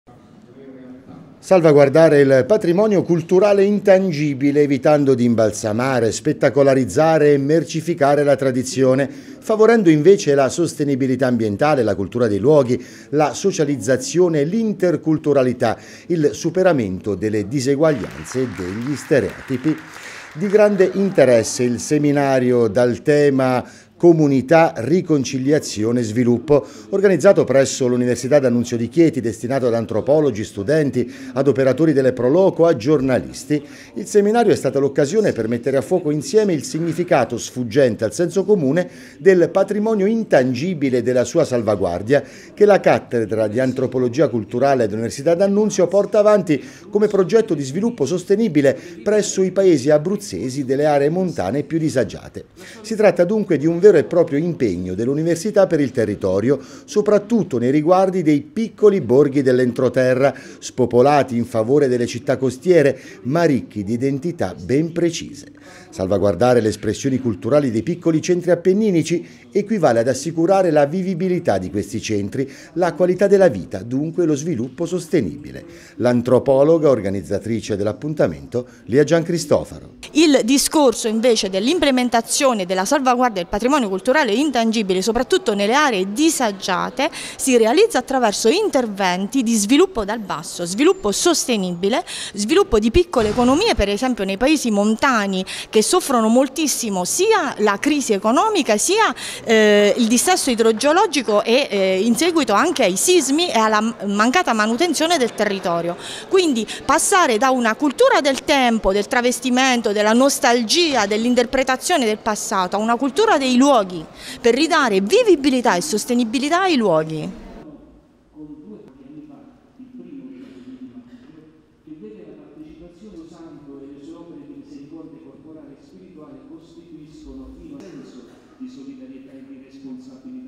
Comunità, riconciliazione e sviluppo
Lanciano, servizio Tgmax 7 dicembre 2016 –  Salvaguardare il patrimonio culturale intangibile delle aree interne superando le disuguaglianze e favorendo la cultura dei luoghi: se ne è parlato in un seminario rivolto ad addetti ai lavori e ai giornalisti nell'aula magna della facoltà di Lettere dell'Università D'Annunzio a Chieti.